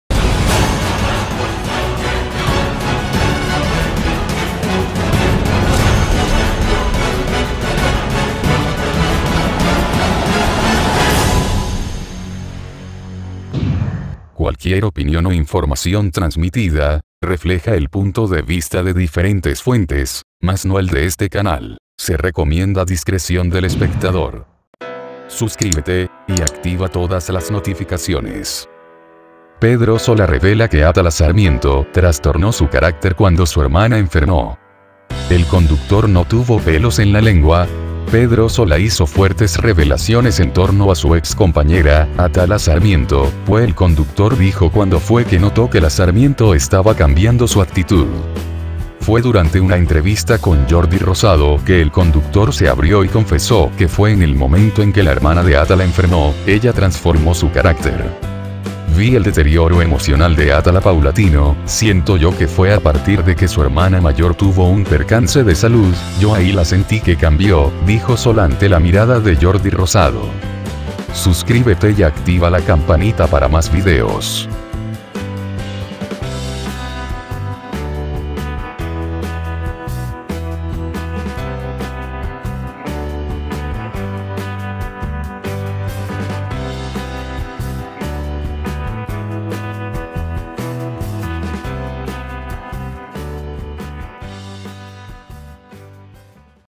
PEDRO SOLA REVELA QUE ATALA SARMIENTO TRASTORNÓ SU CARÁCTER CUANDO SU HERMANA ENFERMÓ
El conductor no tuvo pelos en la lengua...